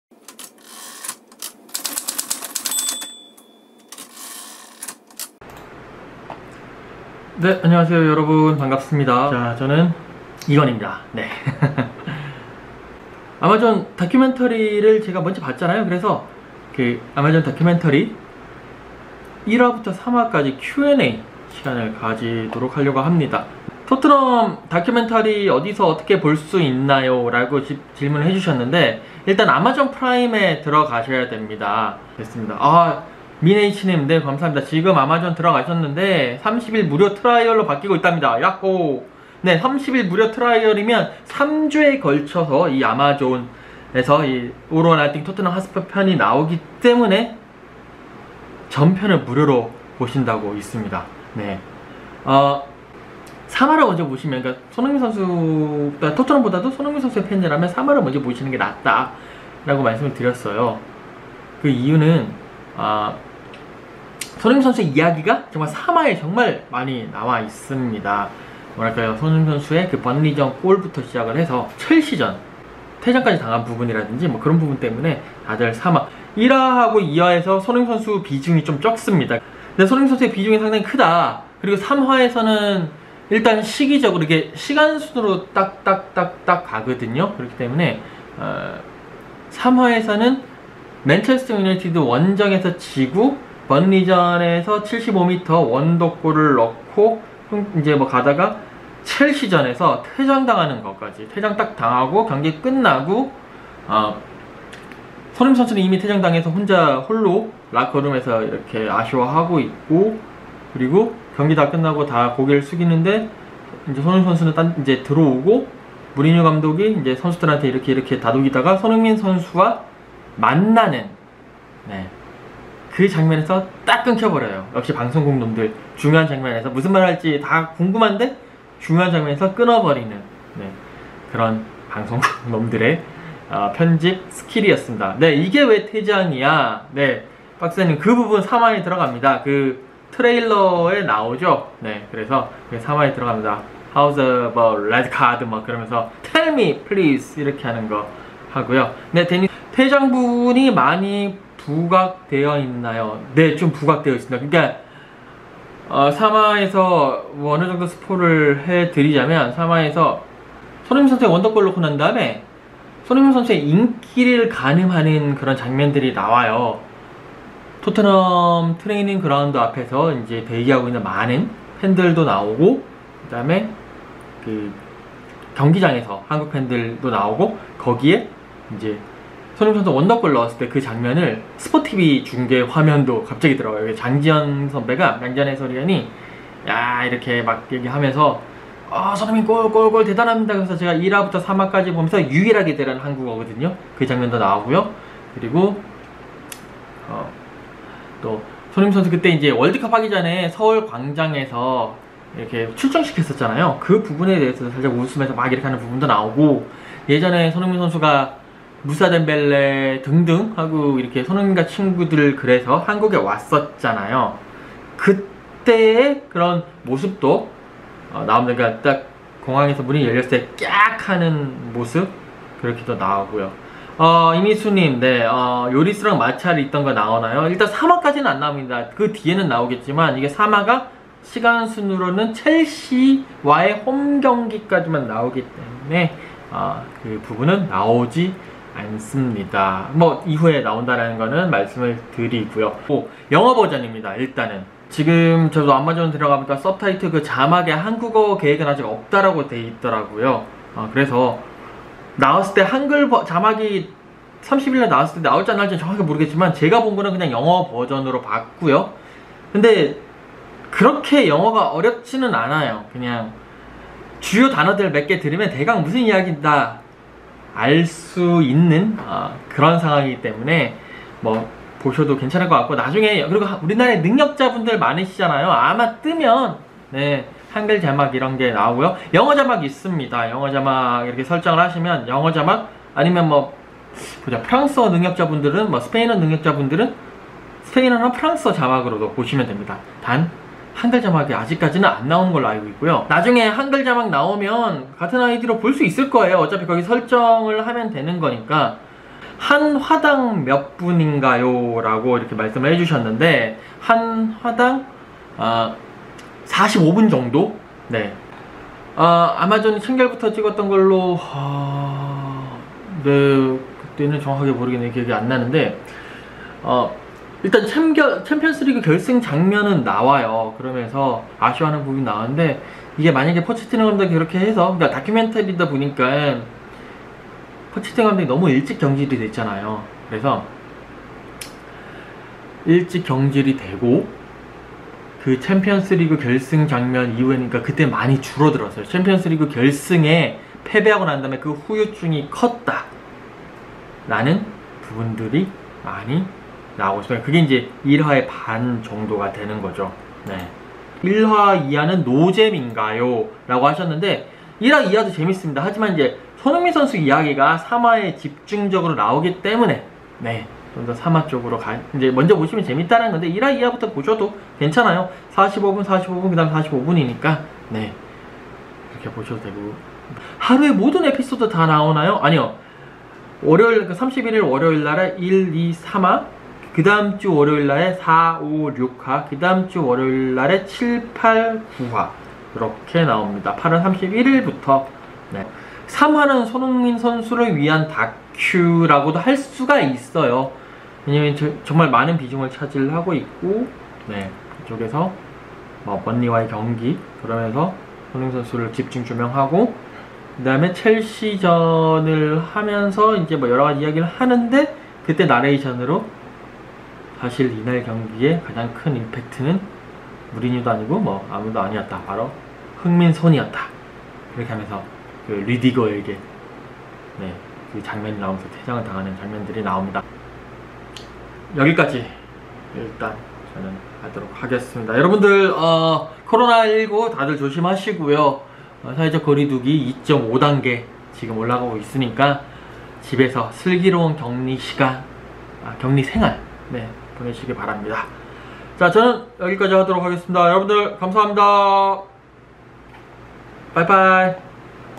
[Q&A]아마존 토트넘 다큐! '한글 자막? 손흥민 비중?' 궁금증 풀어봅니다!
아마존 토트넘 다큐 궁금증 풀어봅니다~~